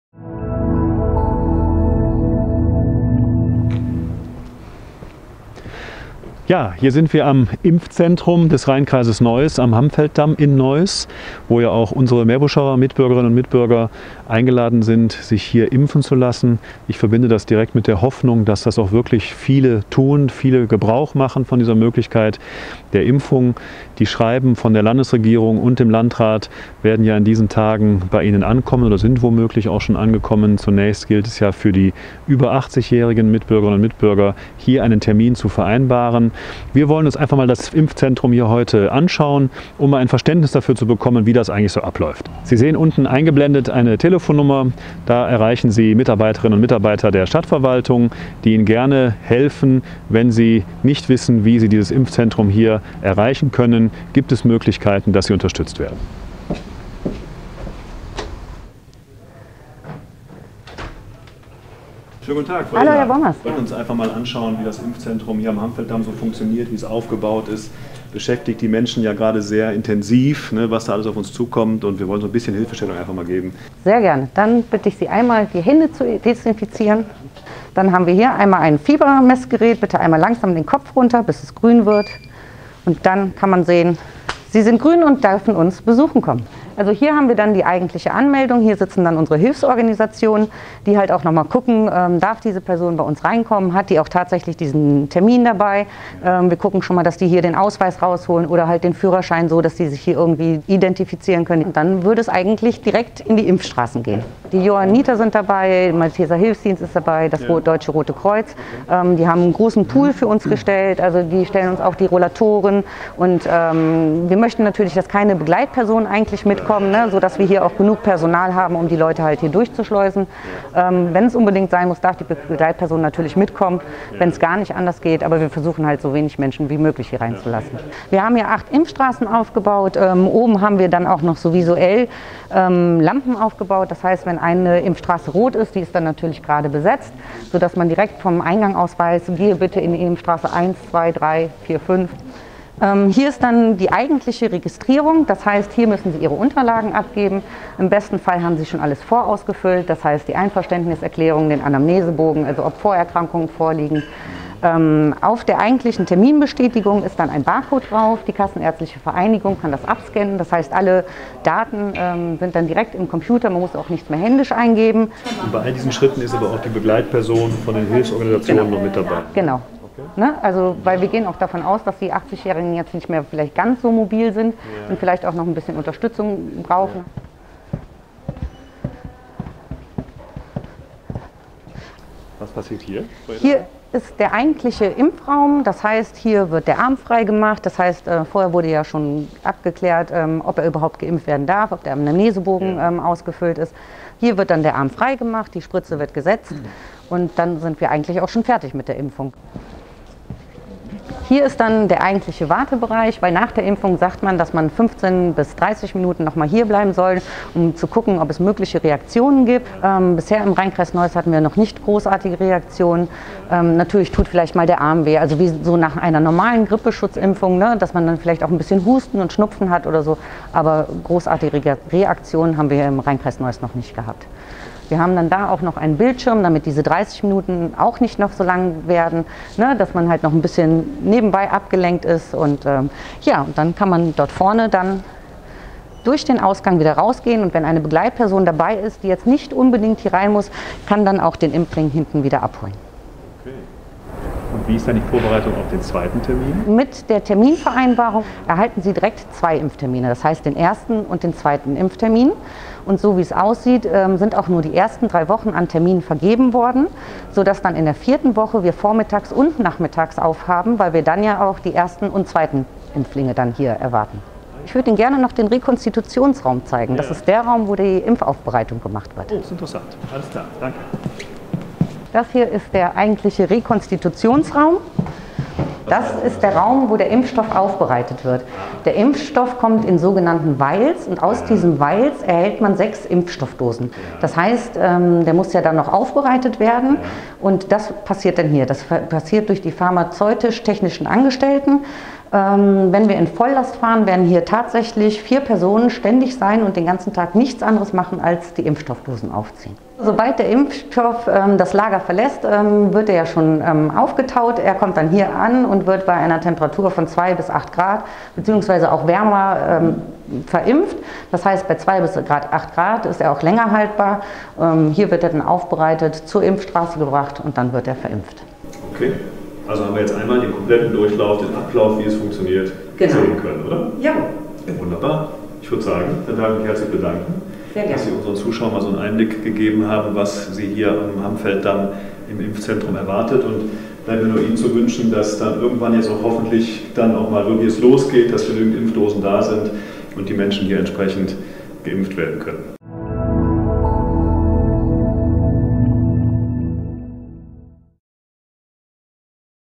Corona-Schutzimpfung: Meerbuschs Bürgermeister Bommers informiert sich über Abläufe im Impfzentrum
Am 8. Februar soll das Impfzentrum des Rhein-Kreises Neuss in der Turnhalle des Berufskollegs für Technik und Informatik auf der Anton-Kux-Straße im Neusser Hammfeld seinen Betrieb aufnehmen. Wie läuft die Impfung vor Ort ab? Was muss ich beachten, und welche Unterlagen muss ich mitbringen? Um Antworten zu liefern, hat sich Bürgermeister Christian Bommers nun selbst einen Überblick im Impfzentrum verschafft.